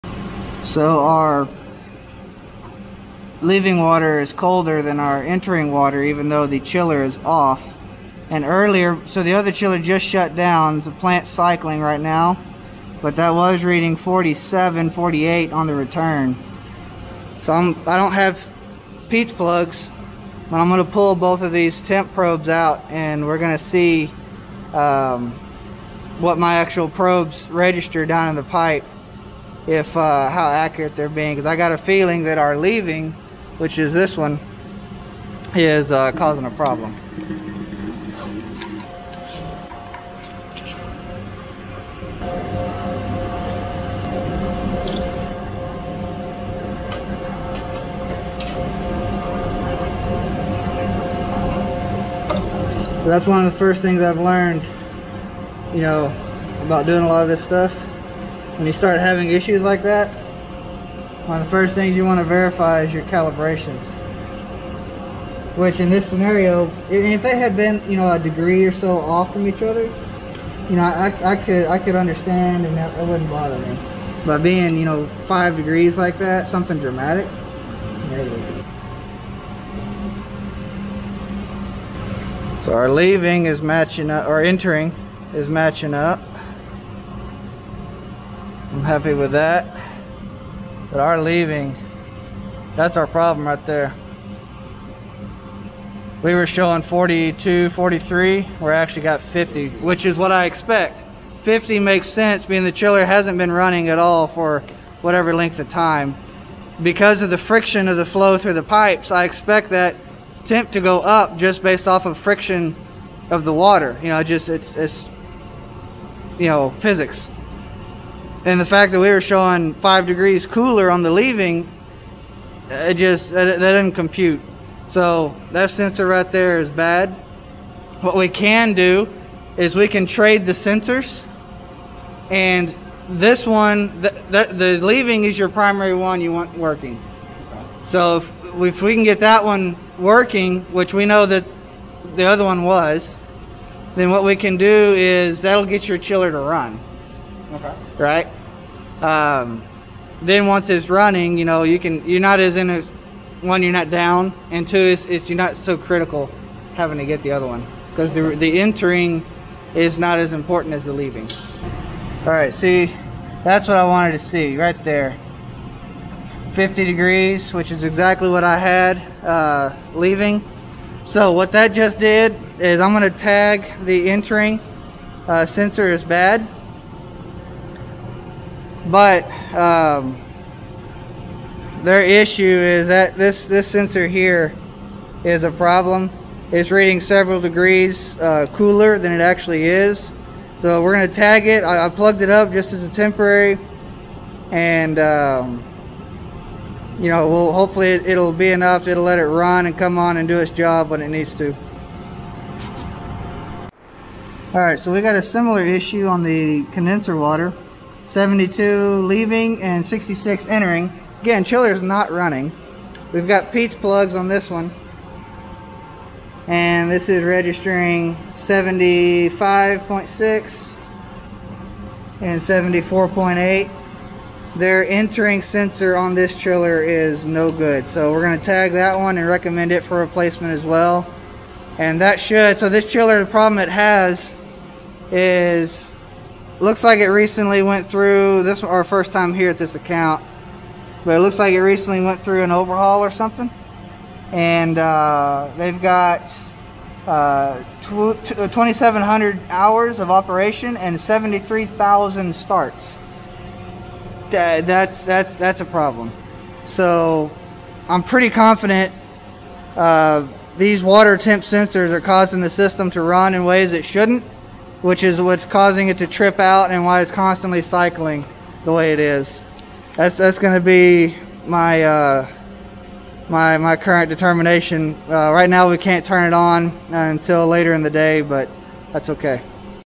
York YS Chiller Bad Sensors
York YS screw chiller has 73k starts but only 2k run hours. Looking into sensors I found both a chilled water and condenser water sensor was out of calibration making chiller think water temp was 8F lower than it really was.